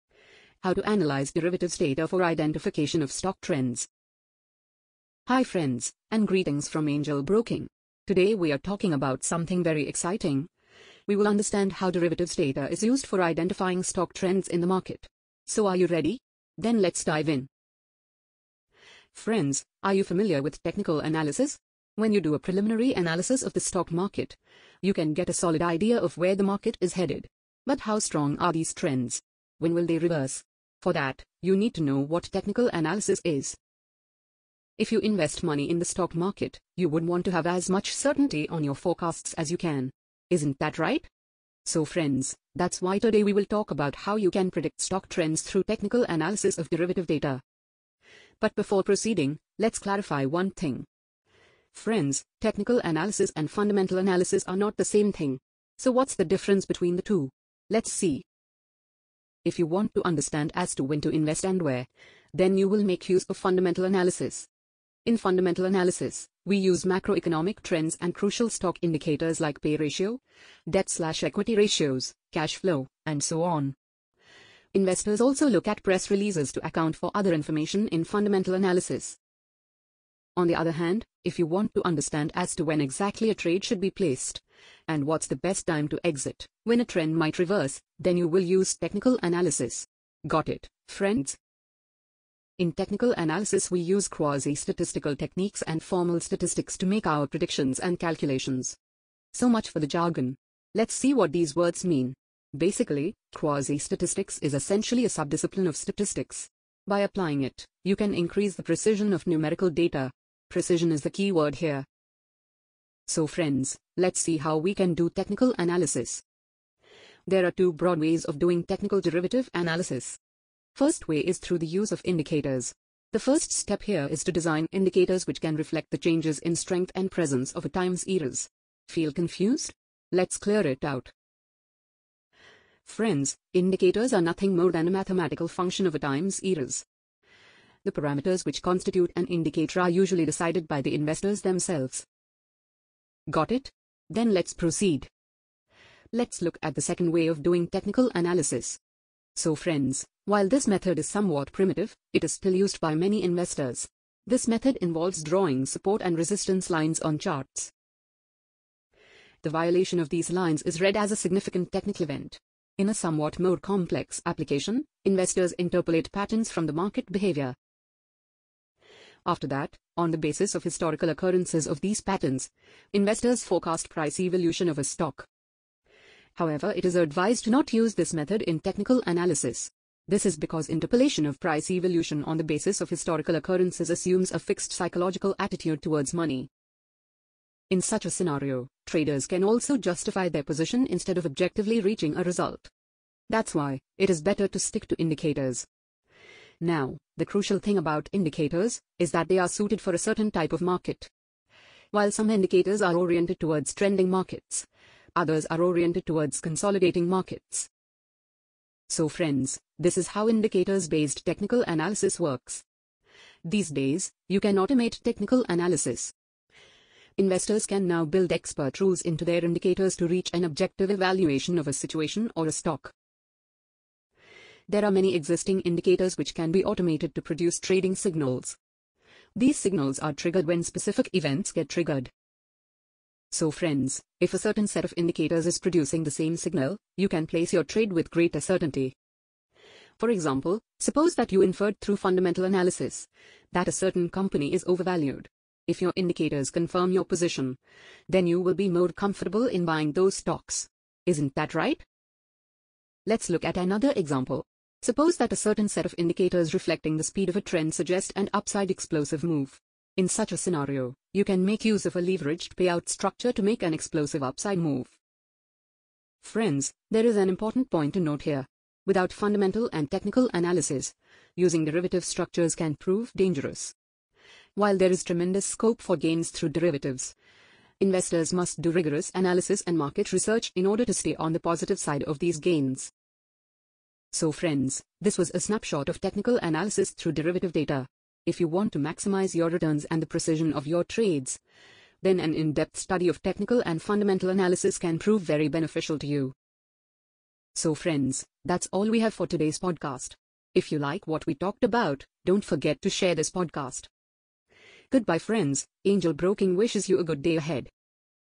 AngelSays Podcast | How to analyses derivatives data | Angel Broking
Investors use Technical Analysis to evaluate the stock market trends and make their decision on investment. Technical derivatives analysis is broadly classified into 2 categories. Want to know more about them? Listen to our latest podcast for more information.

In fundamental analysis, we use macroeconomic trends and crucial stock indicators like PE ratio, debt/equity ratios, cash flow, and so on. Investors also look at press releases to account for other information in fundamental analysis.
On the other hand, if you want to understand as to when exactly a trade should be placed, and what’s the best time to exit, when a trend might reverse, then you will use technical analysis. Got it, friends?
In technical analysis we use quasi-statistical techniques and formal statistics to make our predictions and calculations.
There are two broad ways of doing technical derivative analysis.
First way is through the use of indicators. The first step here is to design indicators which can reflect the changes in strength and presence of a time series. Feel confused? Let’s clear it out.

Let’s look at the second way of doing technical analysis.
So friends, while this method is somewhat primitive, it is still used by many investors. This method involves drawing support and resistance lines on charts.
The violation of these lines is read as a significant technical event. In a somewhat more complex application, investors interpolate patterns from the market behaviour.
After that, on the basis of historical occurrences of these patterns, investors forecast price evolution of a stock.

Stock Market Technical Analysis - 0:21
how you can predict stock trends through technical analysis of derivative data - 0:44
Difference Between technical analysis and fundamental analysis - 0:52
What is Fundamental Analysis - 1:05
What is Technical Analysis - 1:32
how we can do technical analysis - 1:55
use of indicators - 2:00